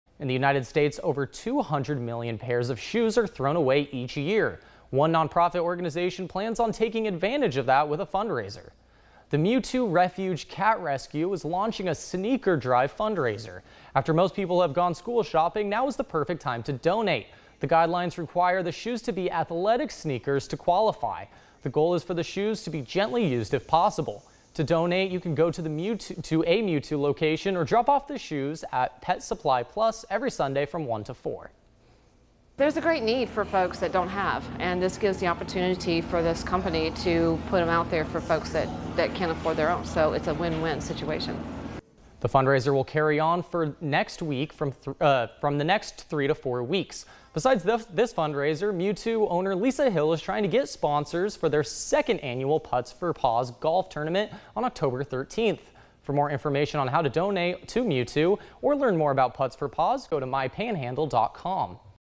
Mew Tew Refuge Cat Rescue launches sneaker drive
The Mew Tew Refuge Cat Rescue is launching a sneaker drive fundraiser.